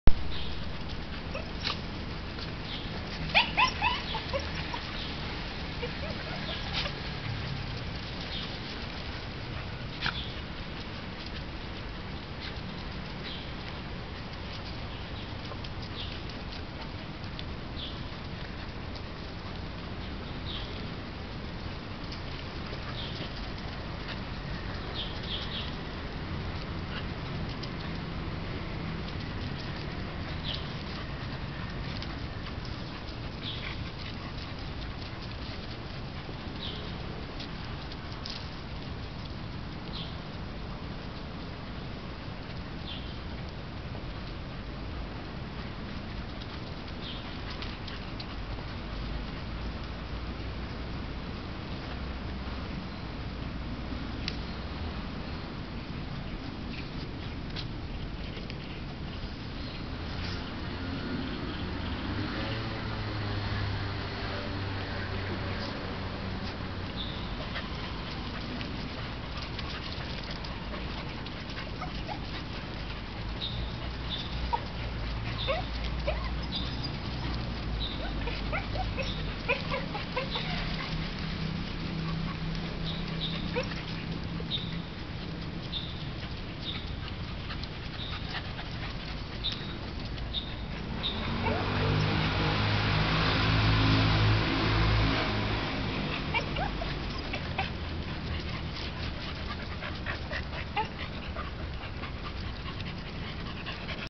8隻狗狗都有看到樹上松鼠蹤跡100825.AVI
佳佳.黑腳蹄.仙草蜜.珠珠.美香.MayMay 大寶  蒜頭一下車到無人公園就發現松鼠